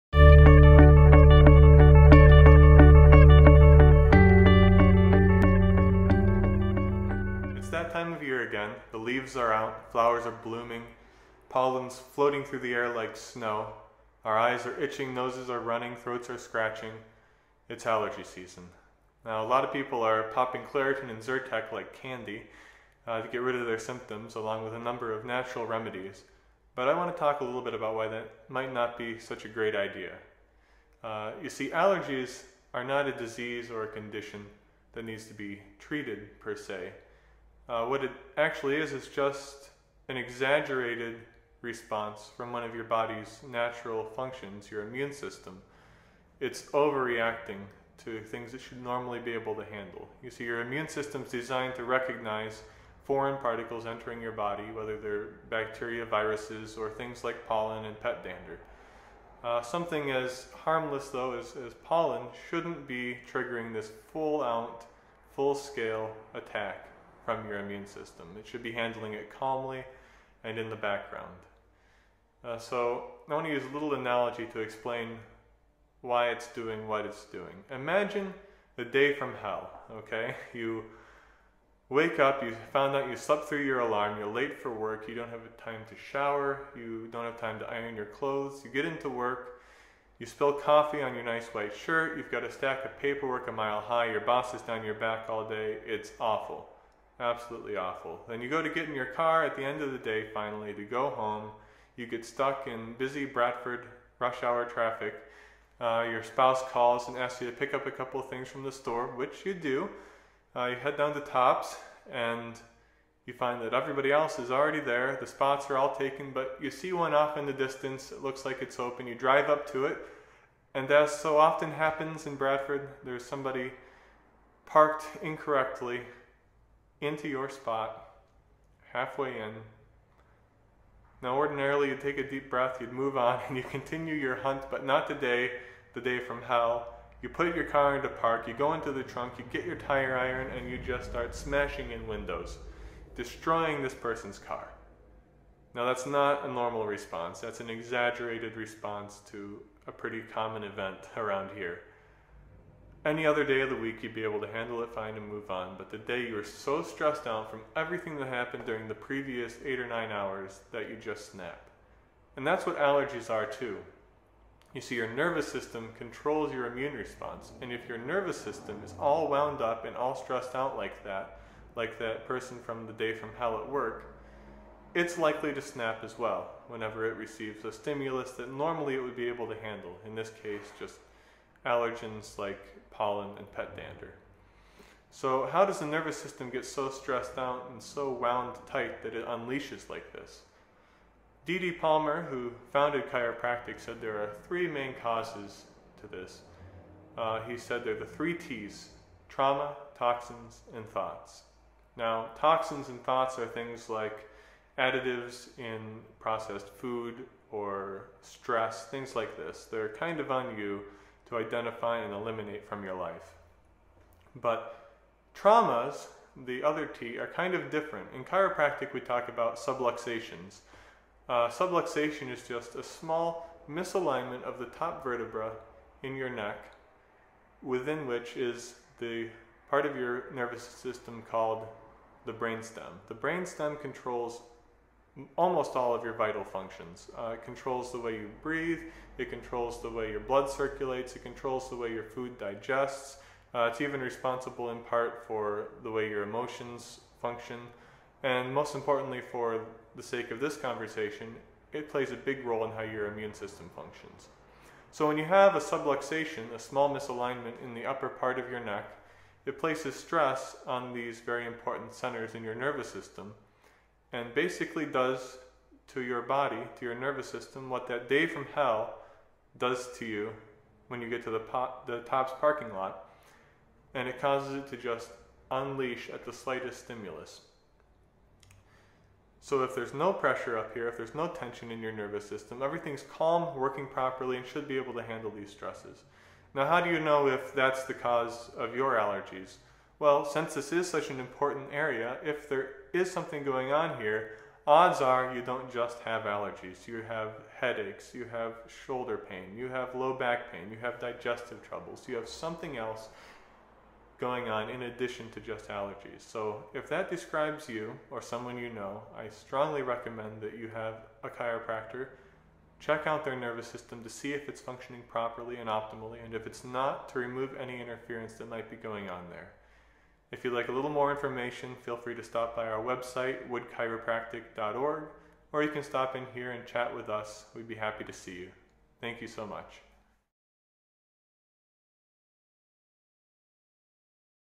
[Tips4Allergies.Blogspot.com] The Truth About Allergies - Dr. Jason Wood
Dr Jason Wood of Wood Chiropractic briefly describes the neurological cause of allergies and what can be done about them.